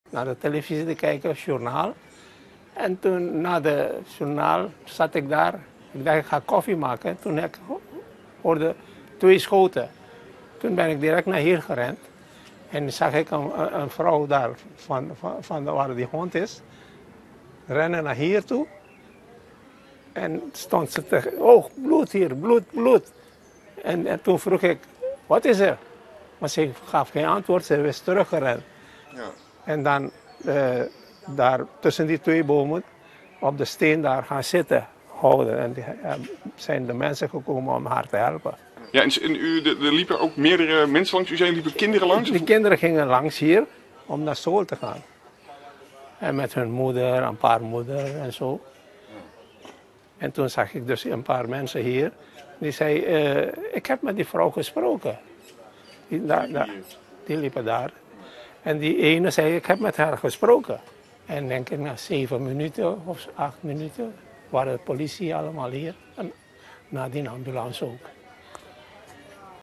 Verdachte aangehouden na schietpartij Koornhorst
De politie heeft donderdagmiddag een verdachte aangehouden na de schietpartij bij de flat Koornhorst. Een arrestatieteam heeft de 37-jarige Amsterdammer uit zijn woning gehaald.